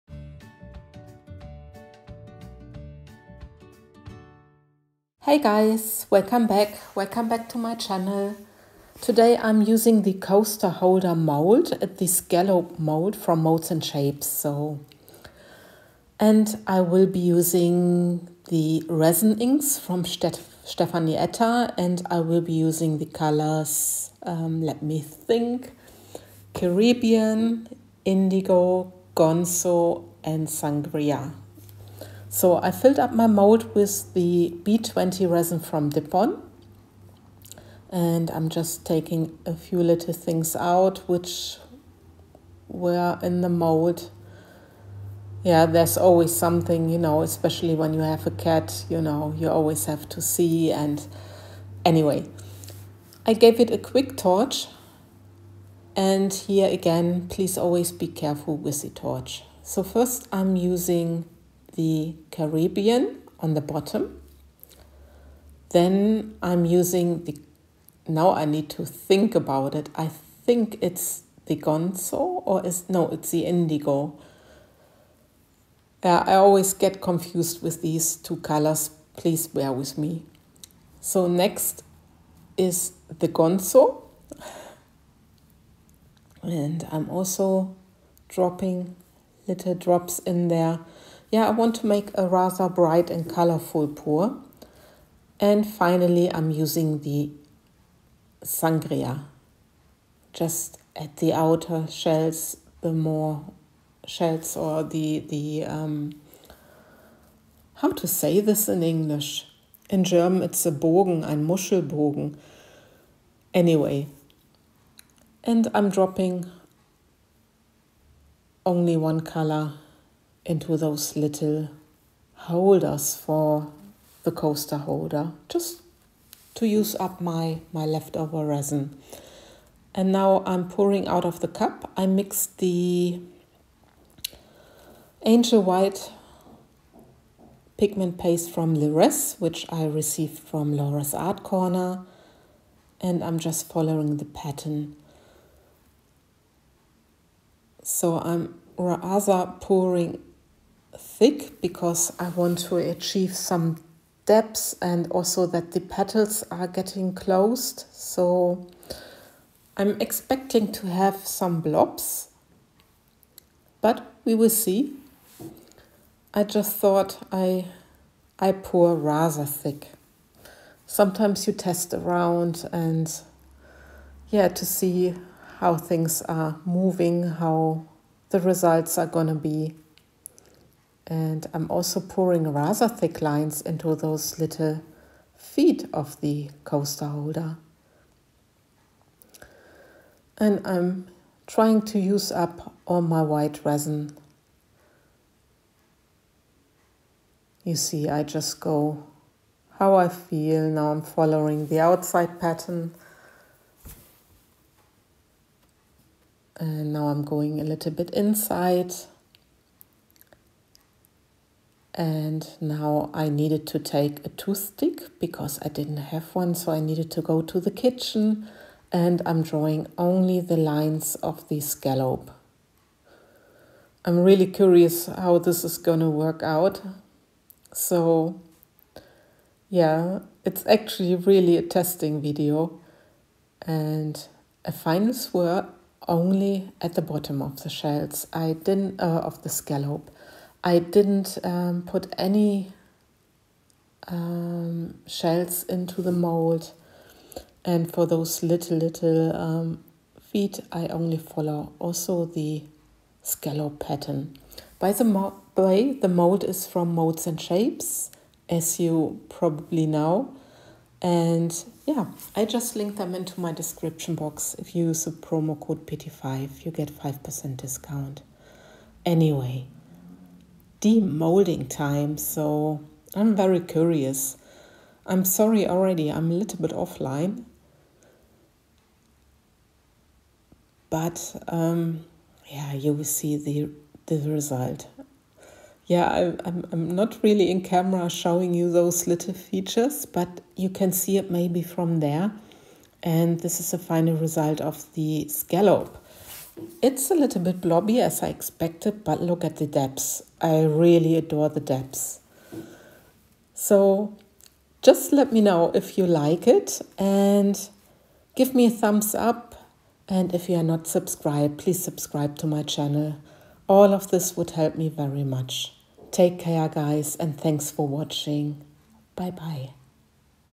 #270 Epoxy Resin: Top or Flop? But the Depth is great 😅 Scallop Coaster
I expected the blobs because I poured in rather thick lines. However, I wonder what you think 🤔 Is it a top or a flop?
I actually love the depth of this piece 😍

I used the Scallop CoasterMold from @moldsandshapes from the Netherlands. All molds they are providing are made on order and 100% handmade and from brillant quality.

Piñata Alcohol Inks